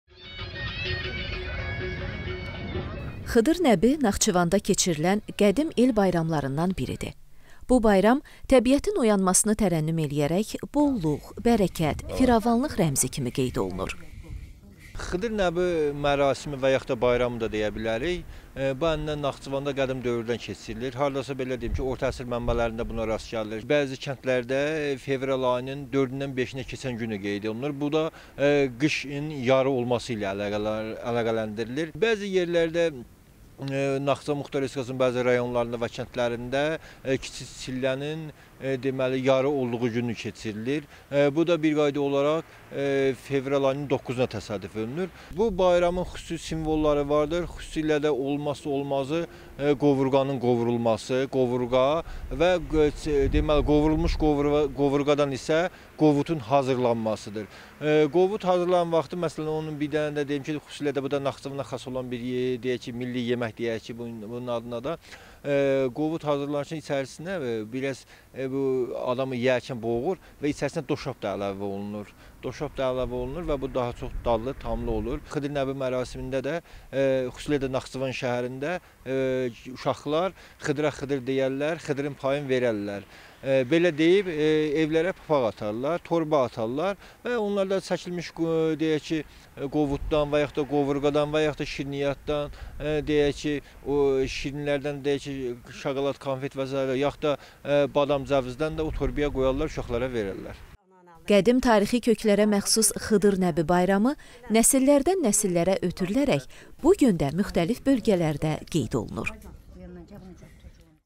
Yazın gəlişini tərənnüm edən bayram | "Xıdır Nəbi" necə keçirilir?
“Xıdır Nəbi” Naxçıvanda keçirilən qədim el bayramlarından biridir.
Bu bayram təbiətin oyanmasını tərənnüm edərək bolluq, bərəkət, firavanlıq rəmzi kimi qeyd olunur.
Tarixçi Asəf Orucov bildirib ki, “Xıdır Nəbi” mərasiminə orta əsr mənbələrində rast gəlinir: “Xıdır Nəbi” bayramı fevralın 9-da Kiçik çillənin yarı olduğu gündə keçirilir. Bu bayramda qovrulmuş buğdadan (qovurğadan) qovut hazırlanır. Qovutu quru yemək çətin olduğundan ona bir az doşab əlavə edirlər”.
Asəf Orucovun sözlərinə görə, “Xıdır Nəbi” bayramında uşaqlar qapılara gedib “Xıdıra xıdır deyərlər, Xıdırın payın verərlər” deyərək papaq atırlar. Torbalara müxtəlif növ şirniyyat, çərəzlər qoyub uşaqları yola salırlar.
Qədim tarixi köklərə məxsus “Xıdır Nəbi” bayramı nəsillərdən-nəsillərə ötürülərək bu gün də müxtəlif bölgələrdə qeyd olunur.